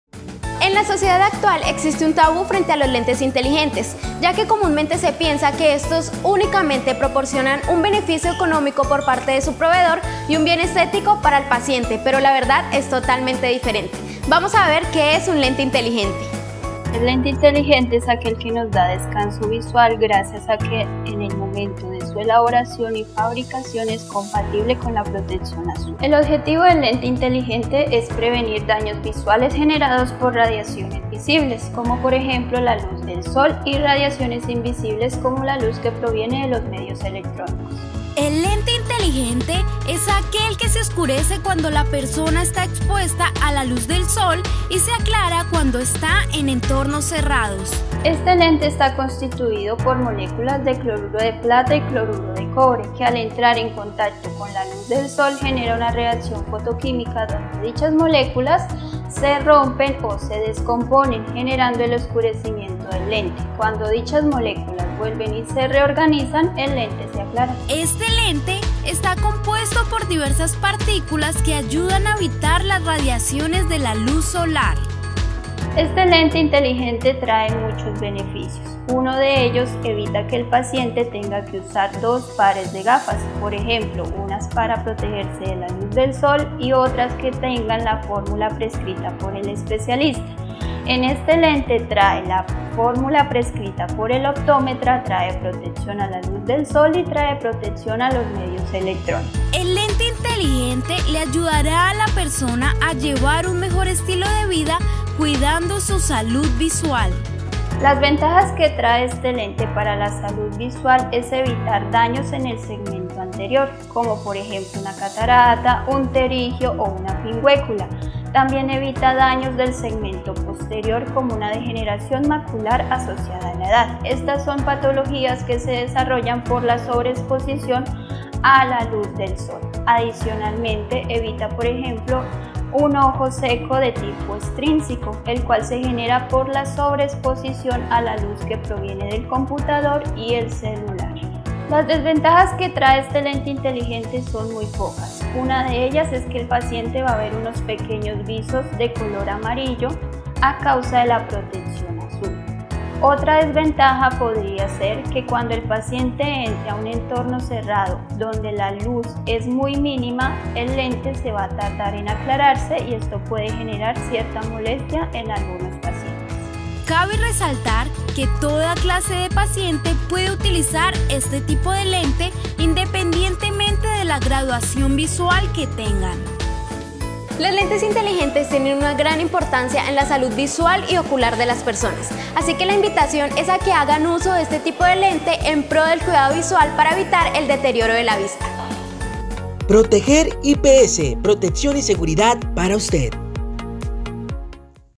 Proteger IPS - Te explicamos para que funcionan los Lentes Inteligentes y su ventajas
¿Te has preguntado cómo sería ver el mundo con una visión mejorada? Descubre los lentes inteligentes en Proteger IPS. Más que solo anteojos, son tu puerta de entrada a una experiencia visual avanzada. Desde mejorar la claridad y nitidez de tu visión hasta sentir la comodidad que te pueden llegar a ofrecer.